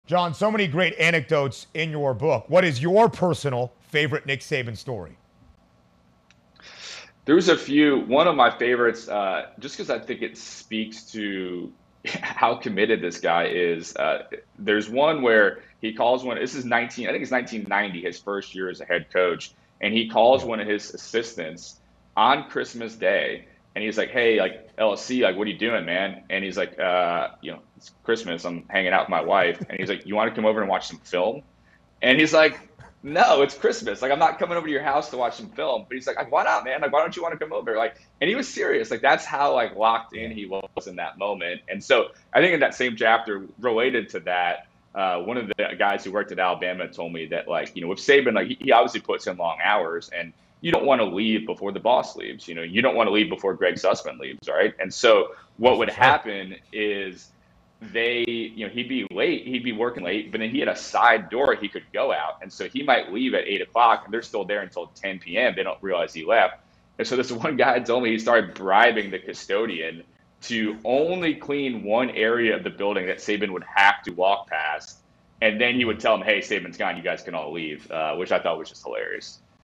A Nick Saban Story You May Not Have Heard Before
John Talty joins Ben to discuss one of his favorite stories from Alabama head coach Nick Saban that he has shared in his new book.